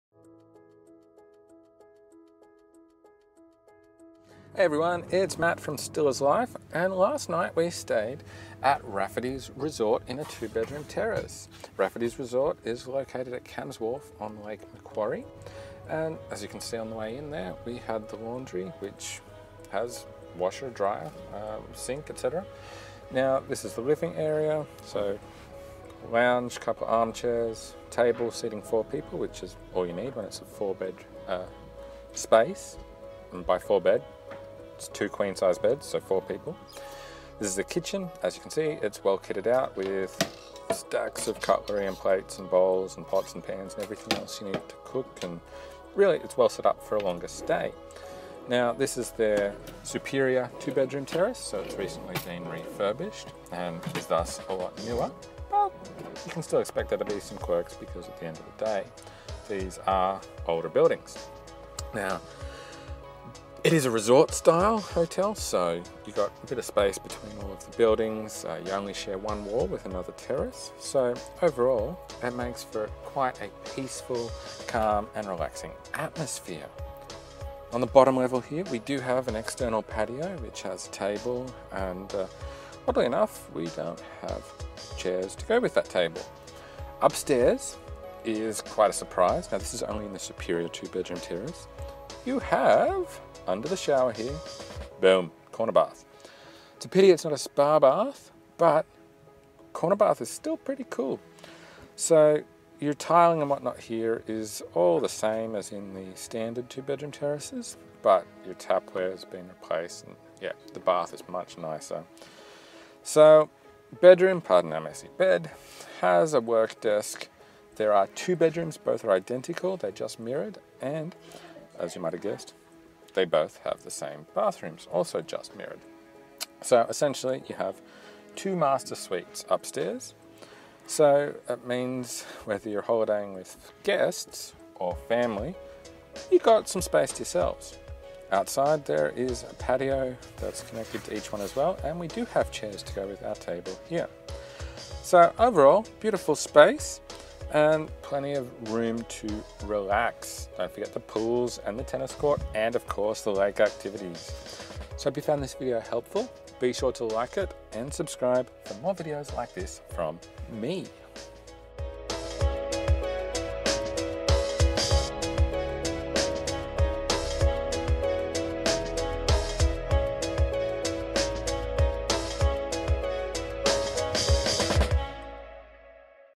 Raffertys Resort - Superior 2 Bedroom Terrace Review - Cams Wharf Lake Macquarie
Raffertys Resort is located at Cams Wharf, Lake Macquarie. It's in a beautiful location with lake views and a relaxed atmosphere. We recently stayed in a superior 2 bedroom terrace, intended for up to 4 guests. Here's what we found!

Gear I use and recommend